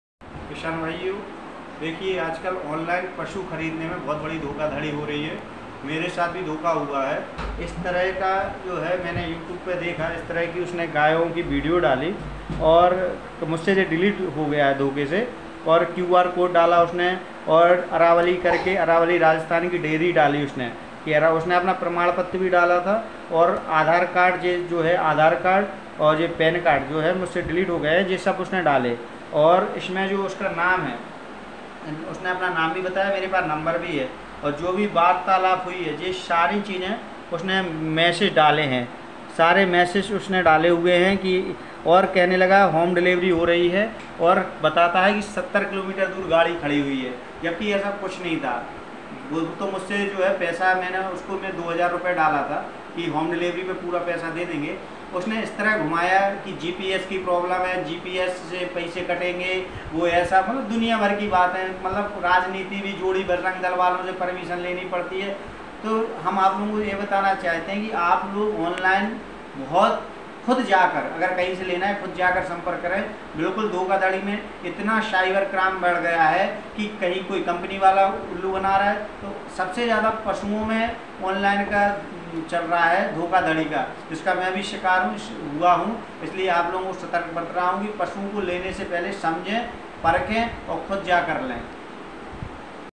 गाय को खरीदने में हो रहा है धोखा मेरी जवानी डिस्क्रिप्शन में पूरी जानकारी डाली हुई है .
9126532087 मोहनलाल वर्मा अपने आप को अरावली डेरी फार्म राजस्थान बताता है पर देरी के नाम पर ठगी कर रहा है डेरी का प्रमाण पत्र डालता है अपना आधार कार्ड पैन कार्ड सब डाल देता है पहले कहता है कुछ पैसे डाल दो फिर बताता है घर से दूर जीपीएस के कारण काम नहीं हो पा रहा है आप पूरी रकम डाल दीजिए और आपकी गए आपके होम डिलीवरी पर पहुंच जाएगी वह पैसे कर कोड फोन पर से ले लेते हैं मैंने उसका नंबर भी इसमें डाल रखा है मेरे मोबाइल पर इस नंबर को डालने पर मोहनलाल वर्मा ही लिखा आ रहा रह था..